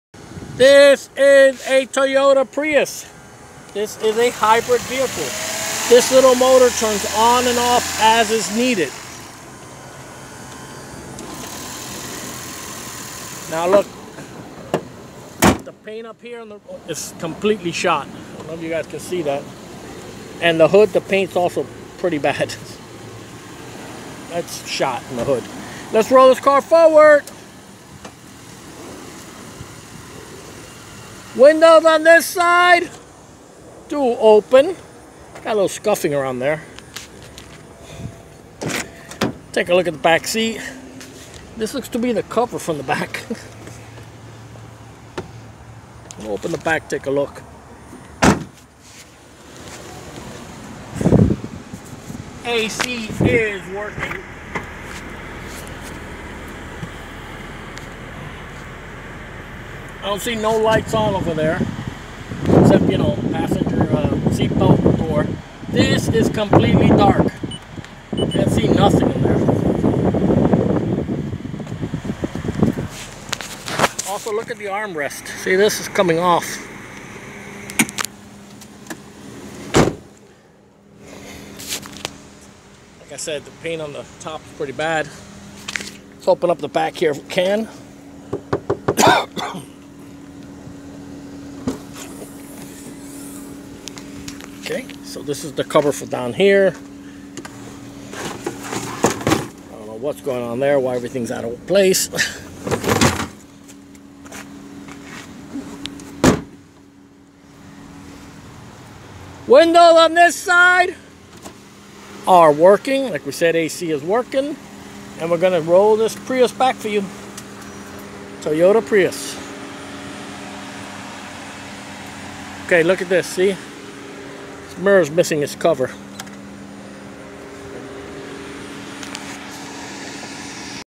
GovDeals: 1430A/ 2007 Toyota Prius 4-Door Liftback
1430A/ 2007 Toyota Prius 4-Door Liftback sold by Miami-Dade County, FL on GovDeals.com